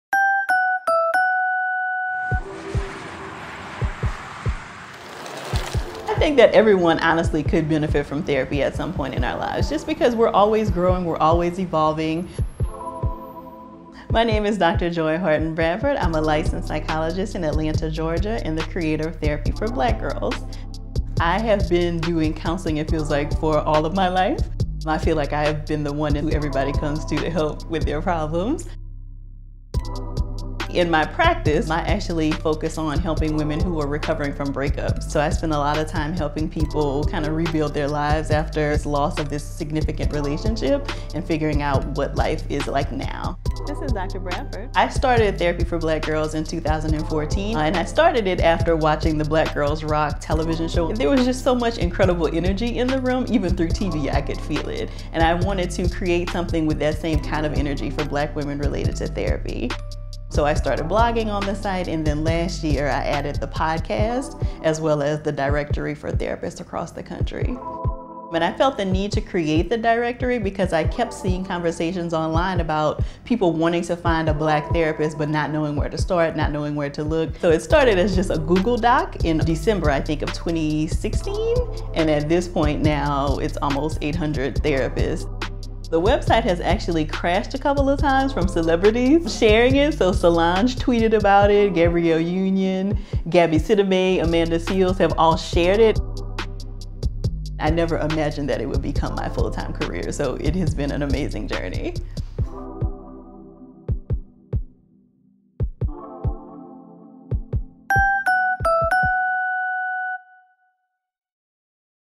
Color Full Lives with Dr. Joy Harden Bradford | State Farm®
Live your most Color Full Life with State Farm. Psychologist Dr. Joy Harden Bradford discusses the creation of a therapist directory and her work helping women overcome breakups through her practice, Therapy for Black Girls.

State Farm. Here to help life go right.